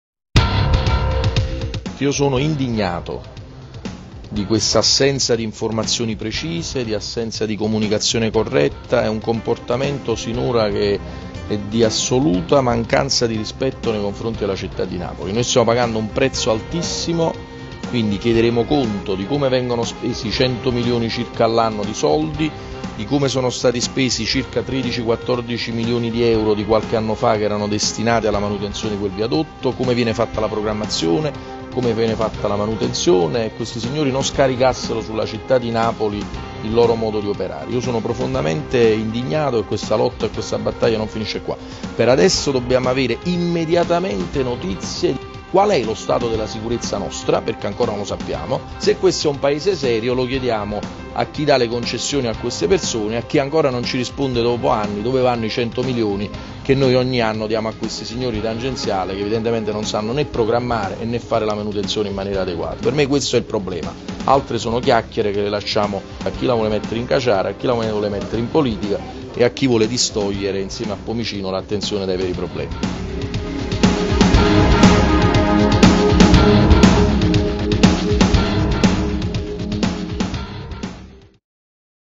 Luigi de Magistris parla dei lavori di manutenzione della tangenziale di Napoli
Il sindaco della città di Napoli Luigi de Magistris parla della vicenda dei lavori di manutenzione della tangenziale  che stanno creando enormi problemi alla mobilità cittadina.